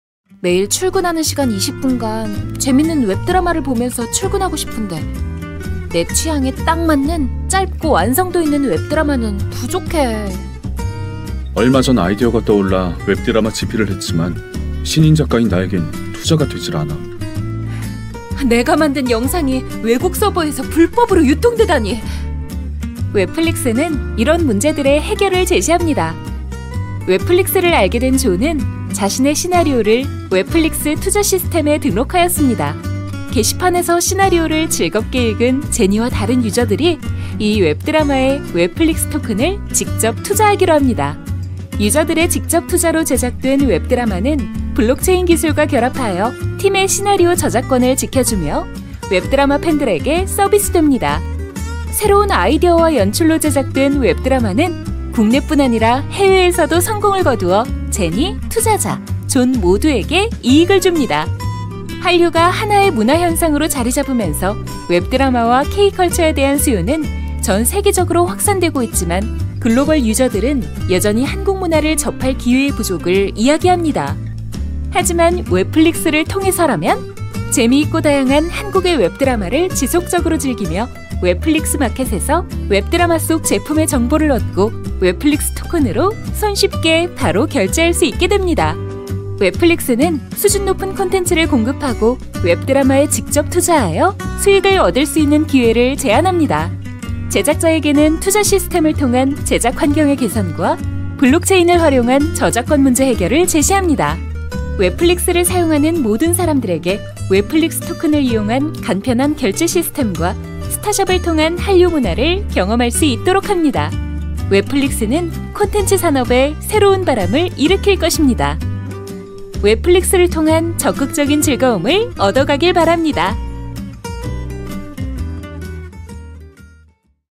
[KOR]웹플릭스 소개 영상 1편_Webflix (WFX)
웹플릭스토큰은 ERC-20을 기반으로 암호화폐 블록체인을 실물 엔터테인먼트 경제와 연결시키는 프로젝트입니다.

블록체인을 활용하여 엔터테인먼트 상품들이 더욱 빠르고 편리하게 국경을 허물고 일반 시청자들이 드라마제작산업에 투자를 하고 수익을 얻어가는 시스템을 제공함으로써 누구라도 참여할 수 있도록 시스템을 만들어 가겠습니다.

그 뿐만 아니라 웹플릭스토큰의 홀더가 되어서 웹플릭스에서 지원하는 서비스상품을 편리하게 이용할 수 있습니다. 웹플릭스토큰은 한국의 문화와 드라마를 전세계적 유저에게 알리는 유용한 수단입니다.